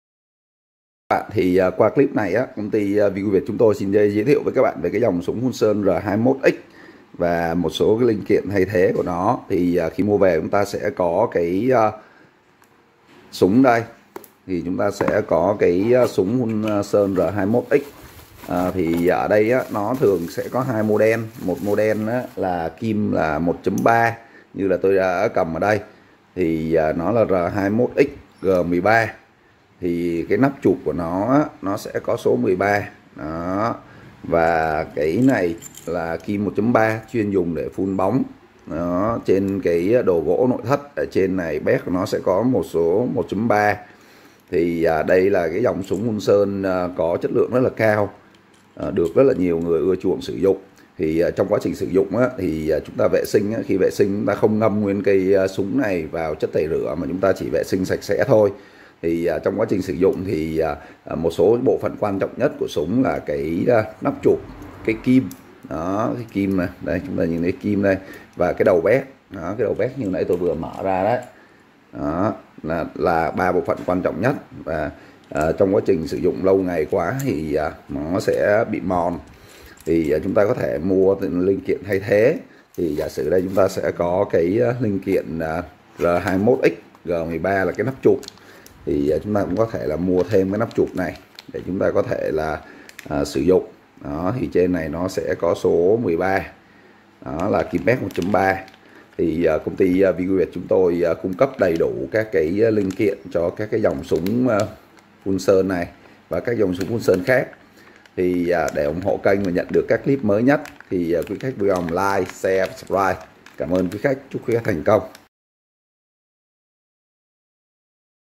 Nắp chụp súng phun sơn Prona R21X-G13 | Nắp chụp súng phun sơn 1.3mm | Linh kiện súng phun sơn Prona
Trong clip này công ty VI GO VIỆT chúng tôi sẽ giới thiệu với quý khách về Nắp chụp súng phun sơn Prona R21X-G13 | Nắp chụp súng phun sơn 1.3mm | Linh kiện súng phun sơn Prona.
CÔNG TY TNHH MTV THƯƠNG MẠI CÔNG NGHỆ VI GO VIỆT
Địa chỉ: 181C, Tổ 2, Khu Phố 3, Phường Long Bình, Biên Hòa, Đồng Nai.
Điện thoại: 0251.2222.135 -  Di động: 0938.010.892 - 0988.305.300